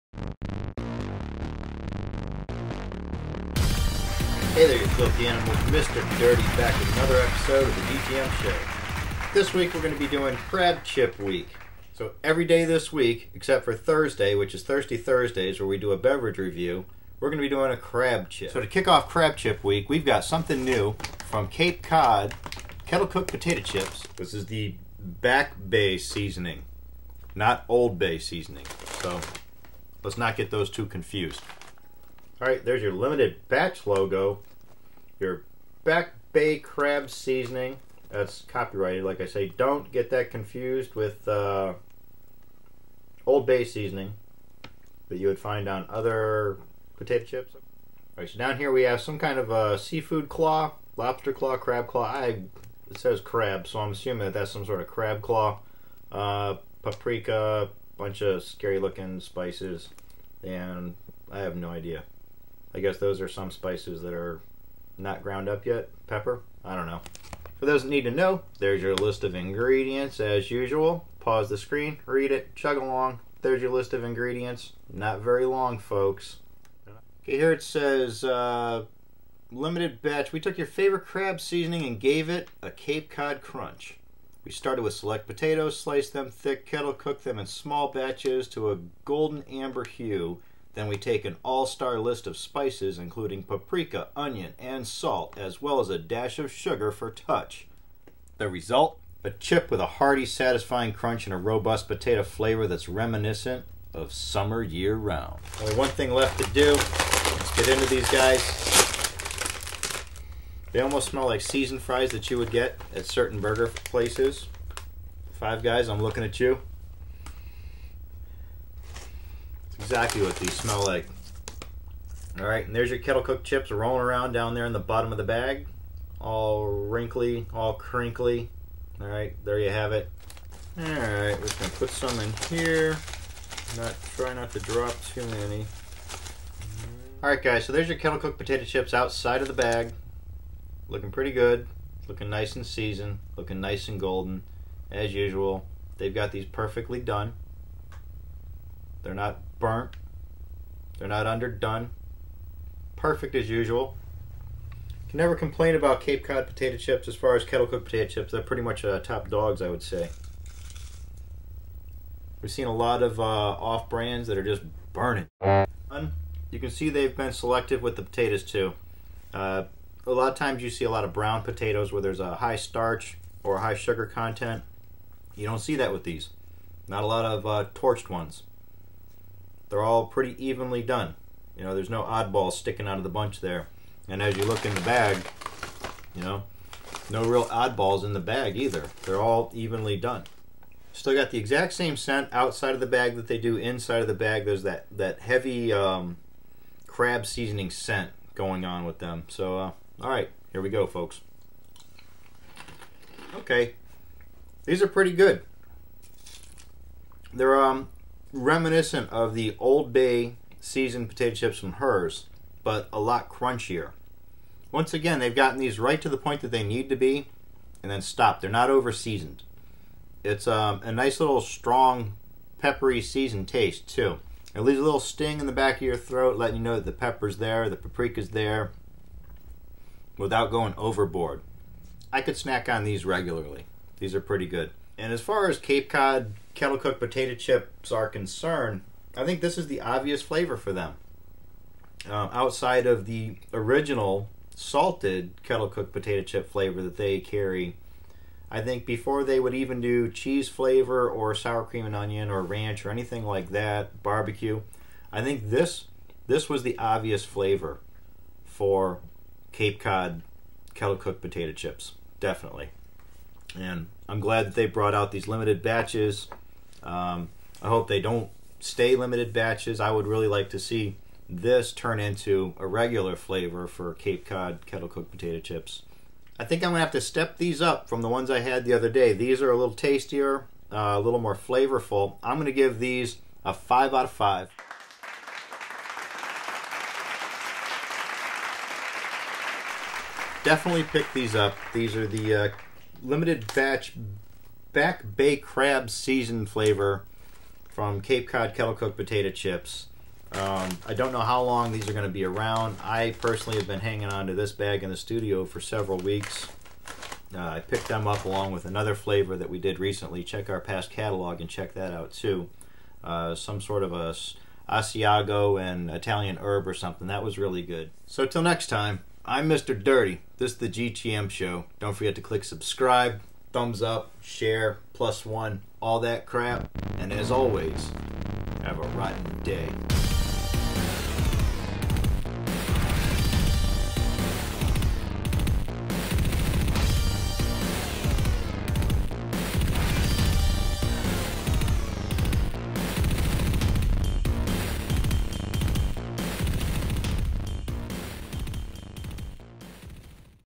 The GTM? Show - LB Cape Cod Back Bay Seasoning Kettle Chips
On this episode Mr. Dirty reviews Limited Batch Back Bay Seasoning flavored kettle chips from Cape Cod.